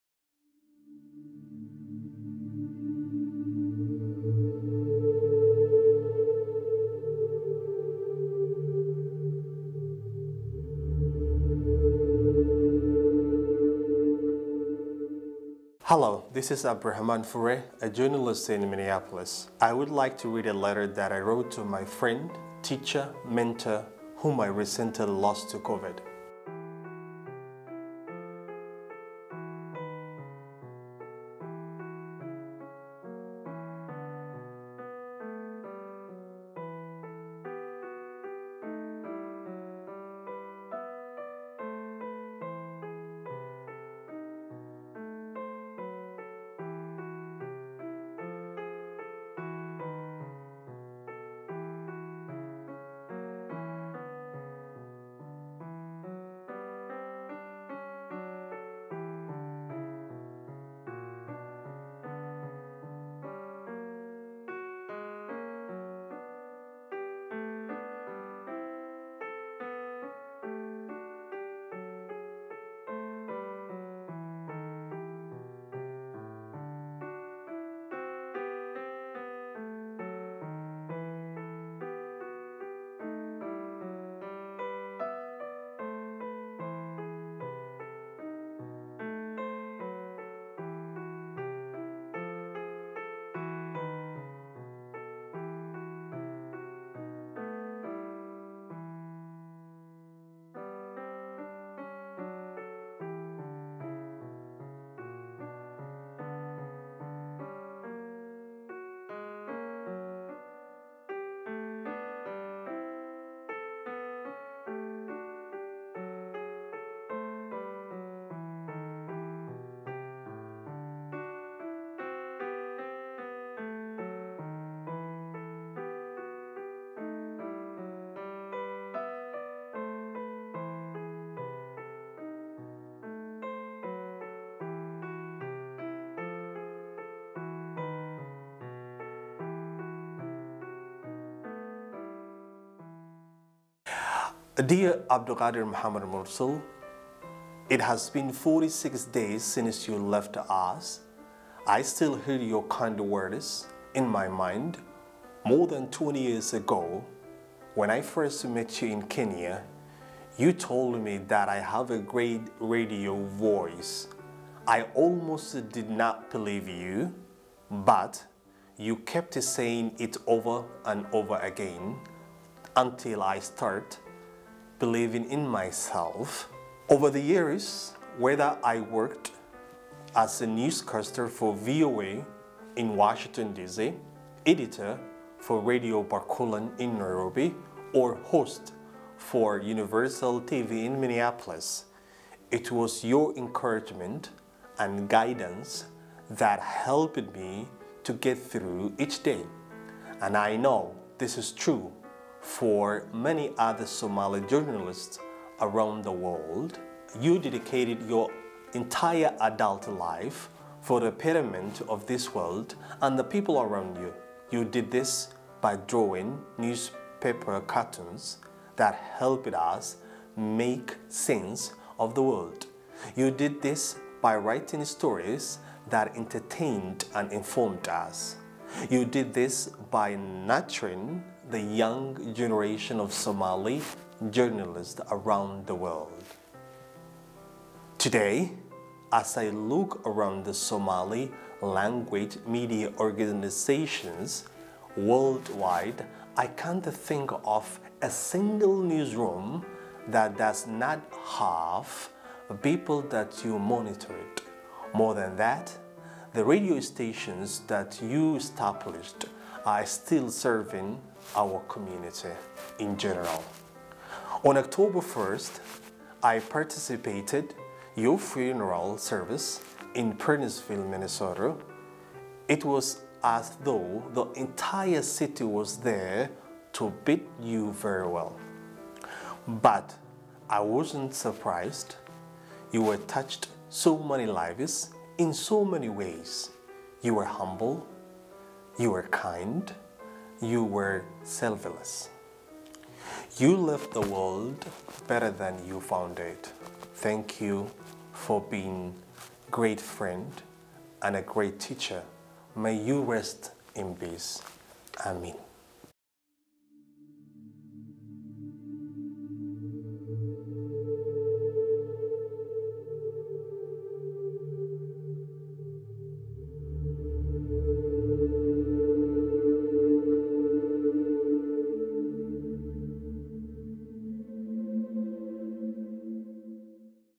Remembering Abdulkadir Mohamed Mursal, a giant figure in modern Somali journalism
A political cartoonist and radio technician, Abdulkadir Mohamed Mursal established radio stations and mentored journalists across East Africa, North America, and Europe.